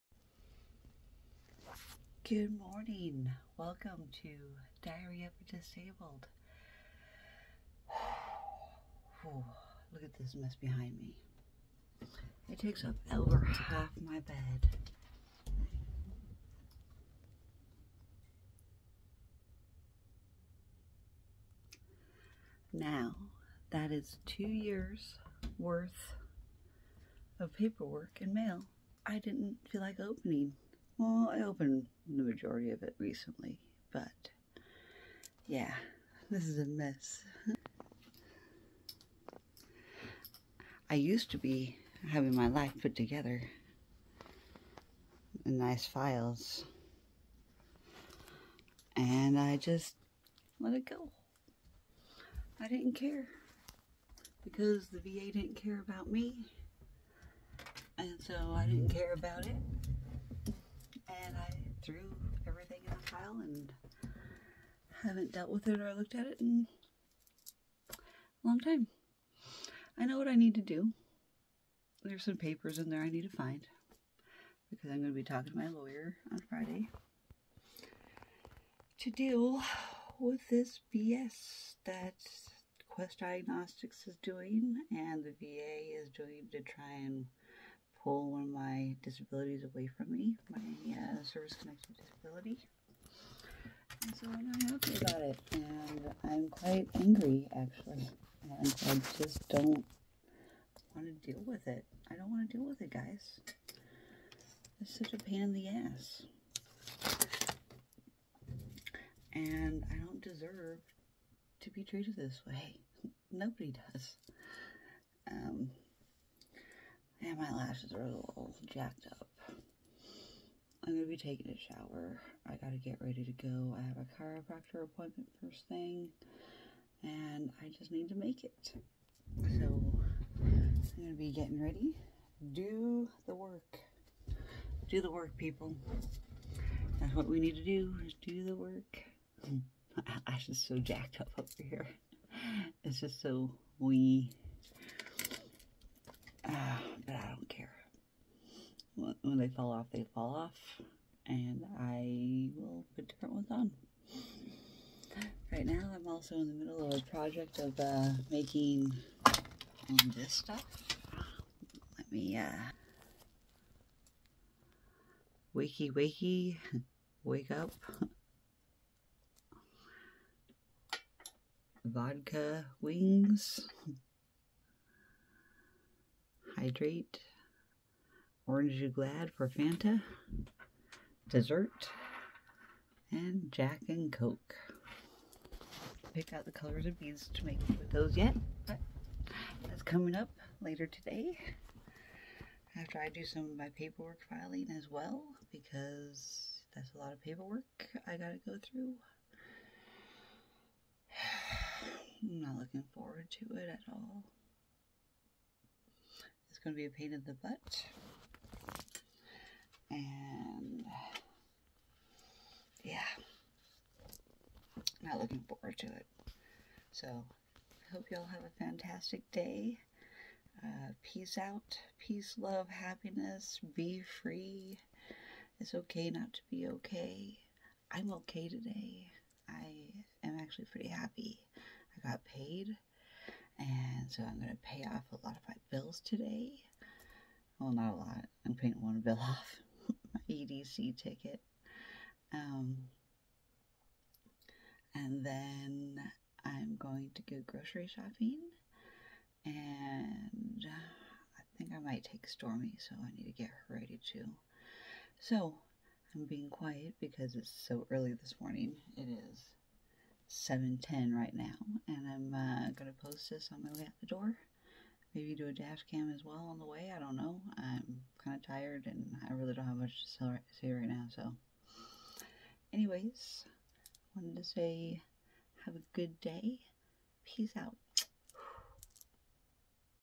Diary of A Disabled: Managing my paperwork today, I used to be very organized, until I said NO!😁😉
I USED to have my life more put together.. However, once the VA stopped caring about me, I stopped caring about my paperwork, over 2 years ago.

Well, NOW it's time I gather my information and start advocating for myself, AGAIN.

I have answers I need to obtain, and I am heading into , hopefully, my last fight for my rights to receive the service connection for a disability that the 9 years  in the Navy, aggravated..

I had NO CLUE I had these "rare" connective tissue issues, that allowed my injuries to go unchecked until my connective tissues gave way to my chronic illnesses deciding to activate.

I can't necessarily prove it, but the Navy/VA cannot disprove it, because I have imaging, proving its a thing.

Unless I get a GOOD NON-BIASED dr, to evaluate me, and LISTEN to my story, On WHY I became disabled IN SERVICE, which ultimately led to my medical discharge.. I will NEVER see justice or retain my 100 percent service connected disabilities..

It's such a looong drawn out fight, that I won already, after 12 years fighting, just to have them (VA), Decide to take away that decision, that they are now saying is erroneous..

I HATE that I have to keep PROVING myself to the VA.. I'm messed up, and they need to pay for it.. I PROUDLY served for 9 years!

I'm happy I did, but my life is now slow and I'm disabled now.. Hook a sista up!!😘